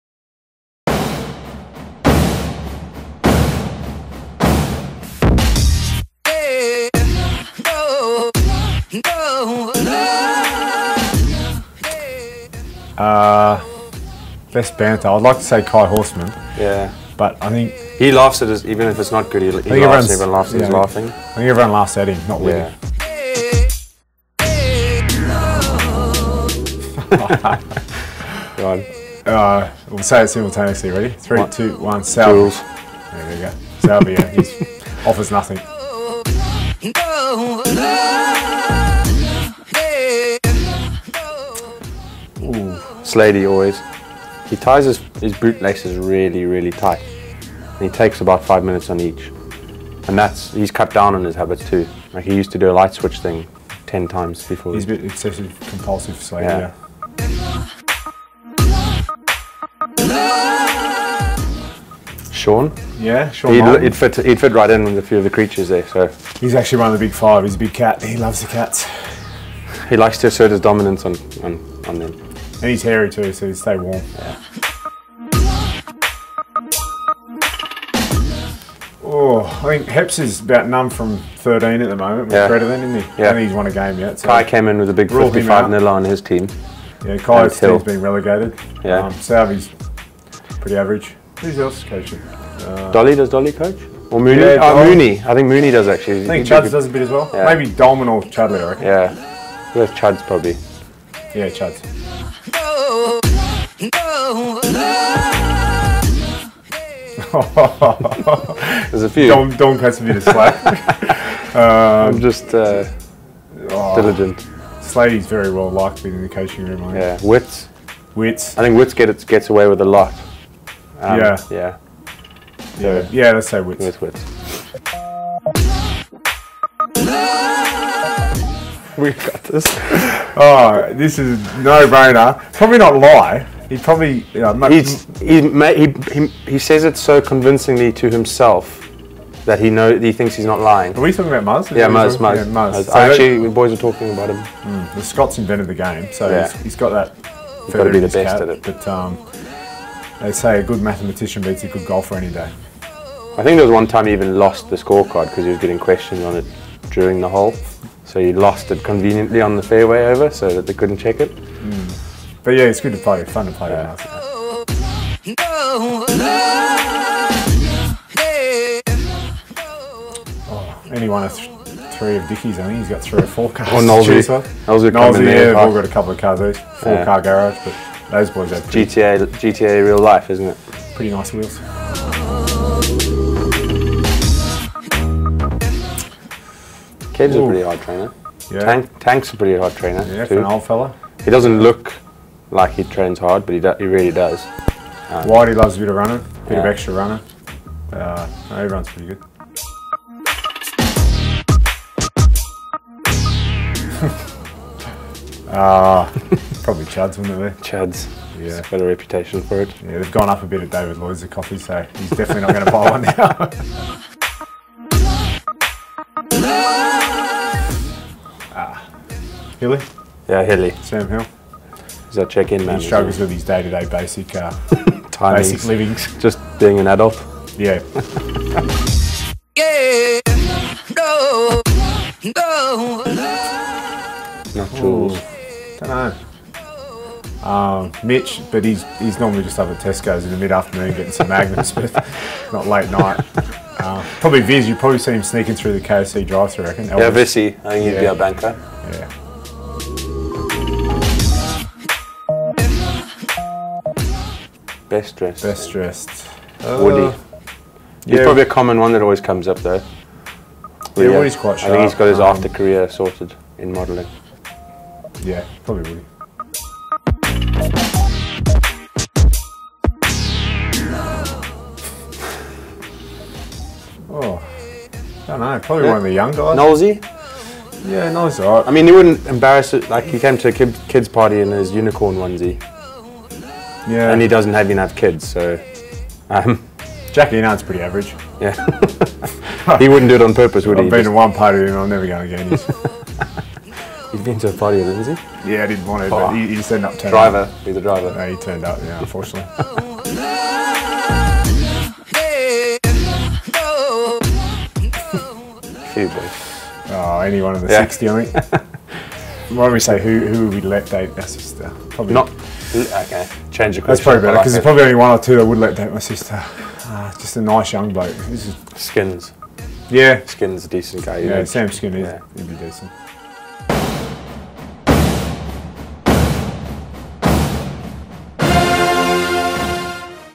Chiefs TV: Team-Mates with Dave Dennis and Don Armand
Exeter Chiefs forwards Dave Dennis and Don Armand dish the dirt on their team-mates at Sandy Park